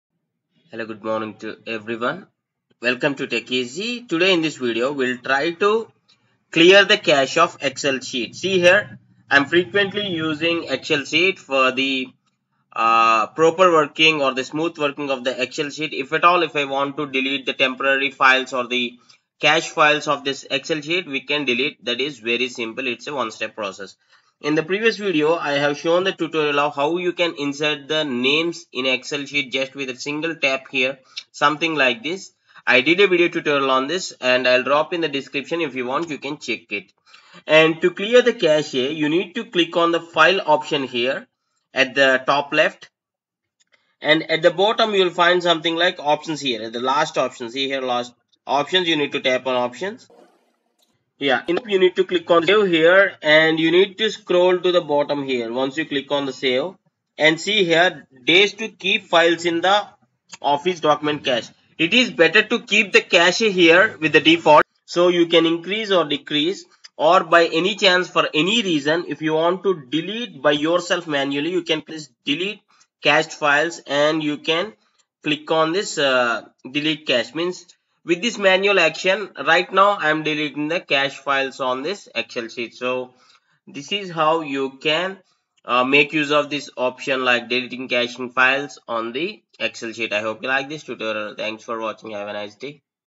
How To Clear Cache In Excel
This is how you can clear the cash of Excel sheet in Microsoft Excel. Even though if you want to change the cache settings in excel, this is the video tutorial you can look after.